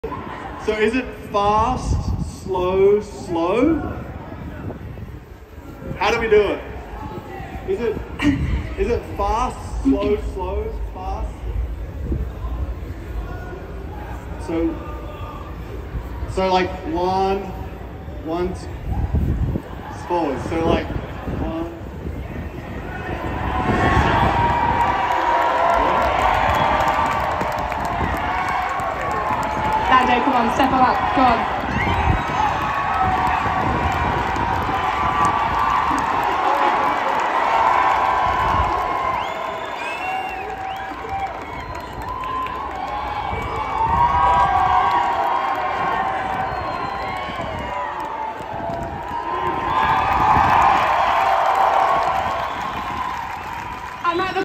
Daniel Ricciardo Dancing With Lando Norris - Texas Two Step Live | US Grand Prix 2022
If you love classic country dancing, then you'll love this video of Daniel Riccardo dancing with Lando Norris. In this performance, the two of them take on the classicTexas two step live at the US Grand Prix in 2022.

If you're a fan of country dancing, then this video is a must-watch! In it, you'll see Daniel Riccardo and Lando Norris dancing to some of the classic country songs that you love. From George Jones to Taylor Swift, you'll be able to enjoy their performance live on stage at the US Grand Prix in 2022.

▶ Become a Channel Member (coming soon)
▶ Leave a super thanks

* Channel Members and Donors will be listed at the end of the video